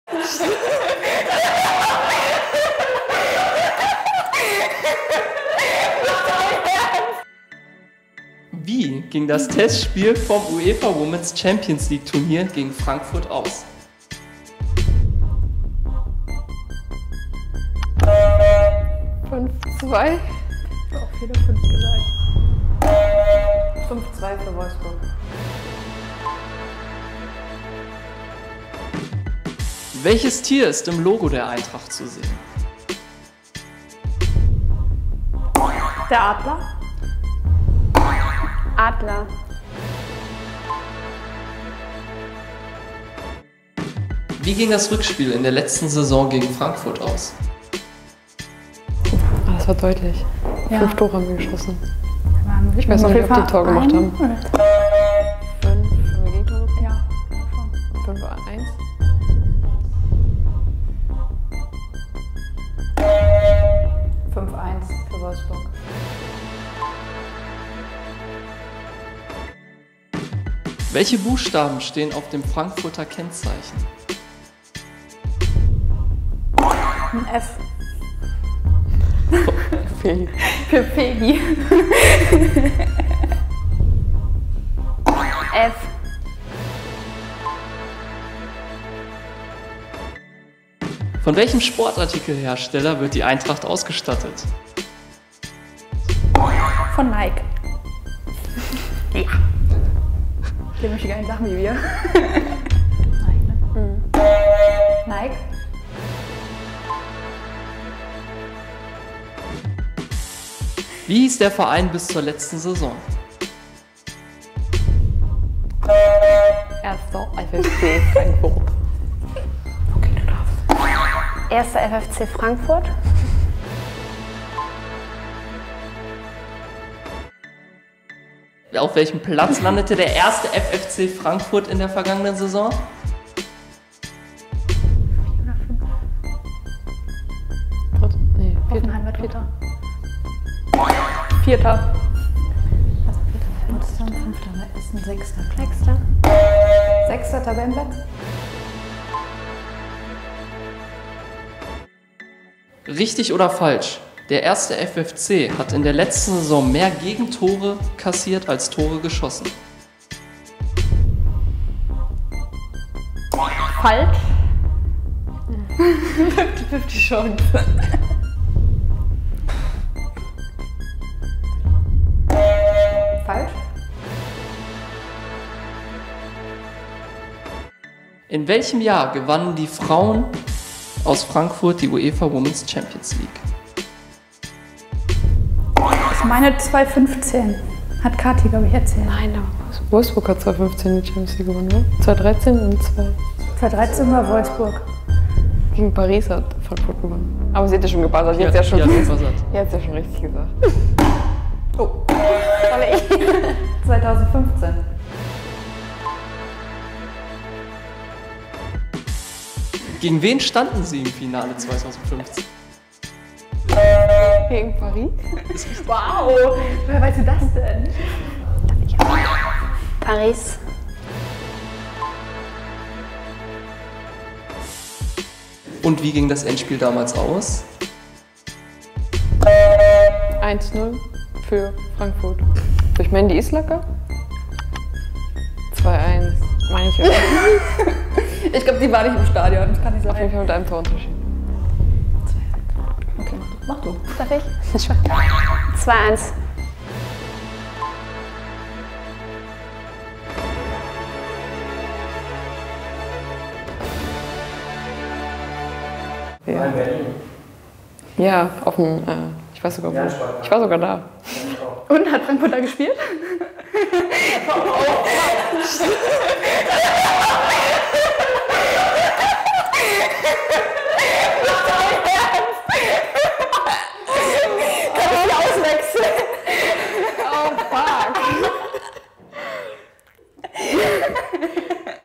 Rauch & Wolter vs. Huth & Hendrich | SPIELTAGSQUIZ vor VfL Wolfsburg - Eintracht Frankfurt
Unsere Wölfinnen wollen wieder siegen und treffen am 10. Spieltag der FLYERALARM Frauen Bundesliga auf die Eintracht Frankfurt Frauen, vormals 1. FFC Frankfurt. Im Spieltagsquiz, präsentiert von Collonil, duelliere sich die beiden Teams Felicitas Rauch & Pia-Sophie Wolter und Svenja Huth & Kathrin Hendrich gegeneinander an! Wer kann sich durchsetzen und weiß mehr über die Duelle der VfL Wolfsburg Frauen und dem 1. FFC Frankfurt!